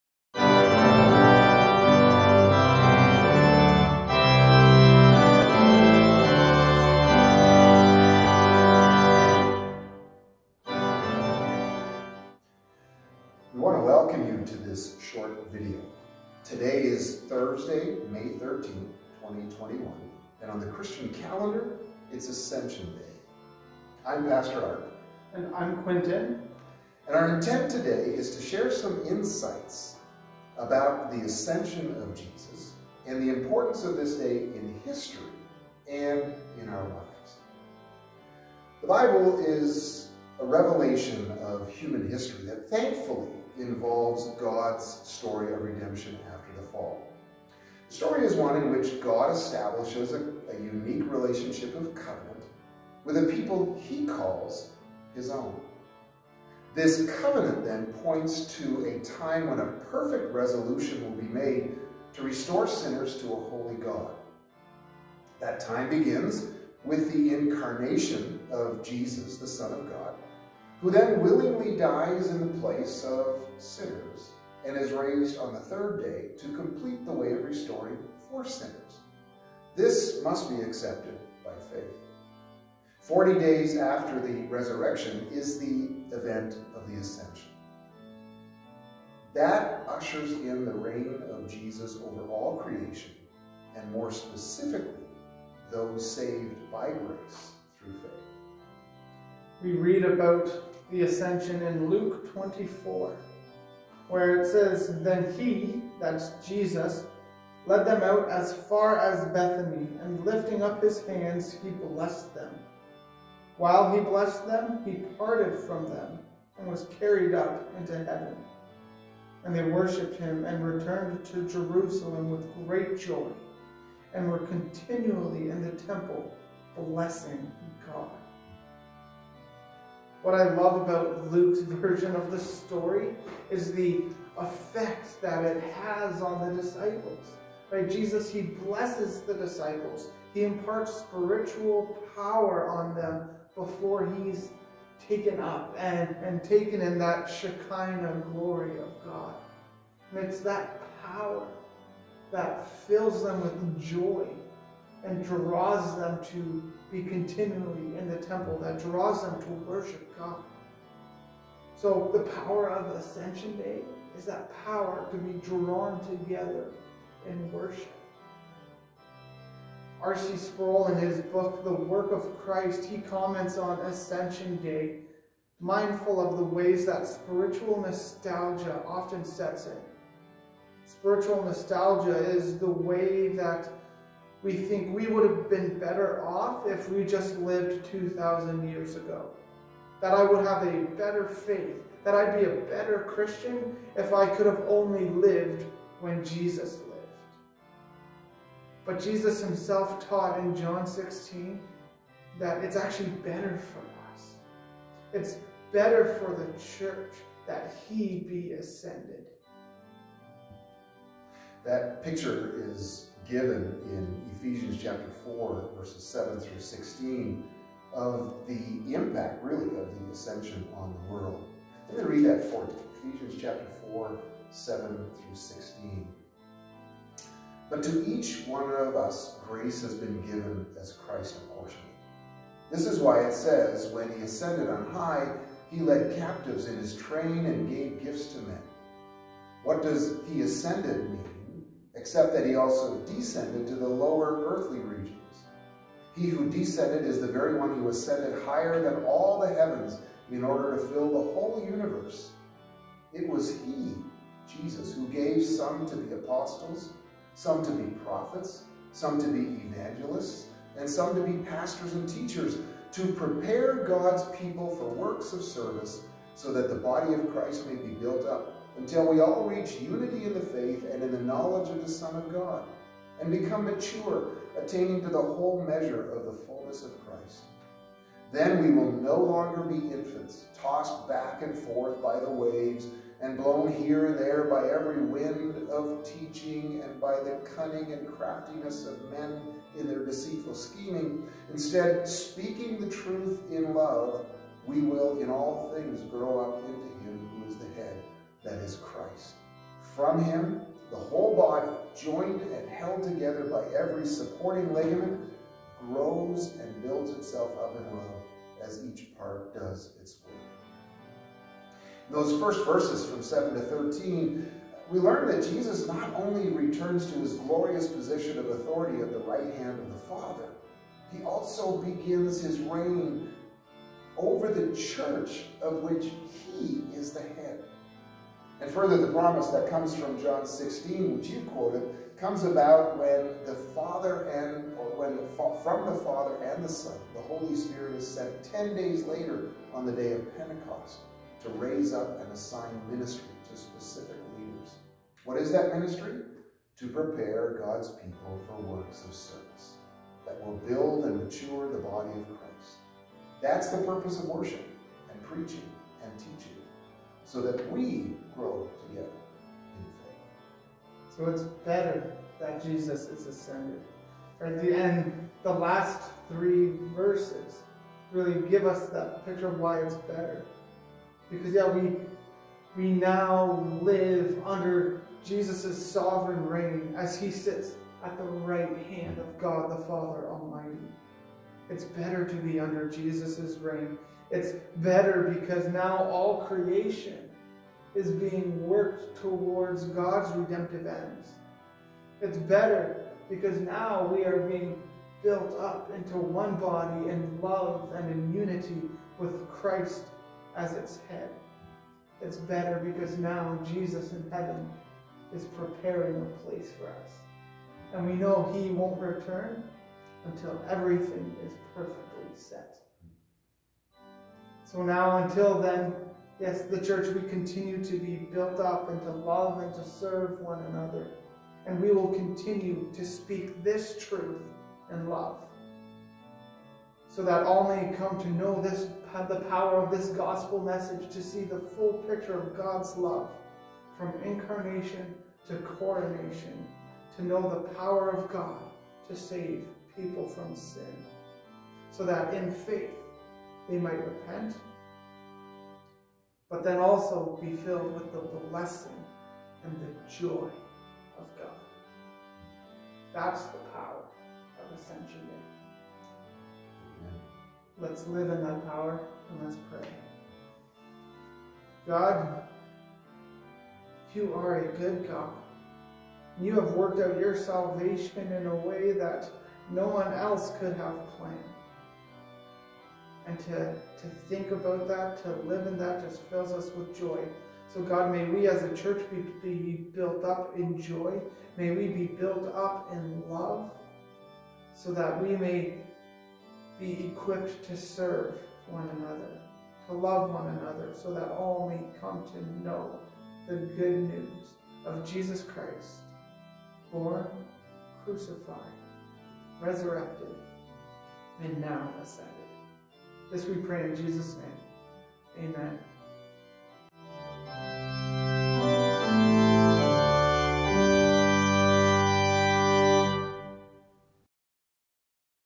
Ascension Day teaching
May 13, 2021 is Ascension Day in the Church Calendar. Hear Pastor Art & Quinton talk about why this day is so important for the life & mission of the church.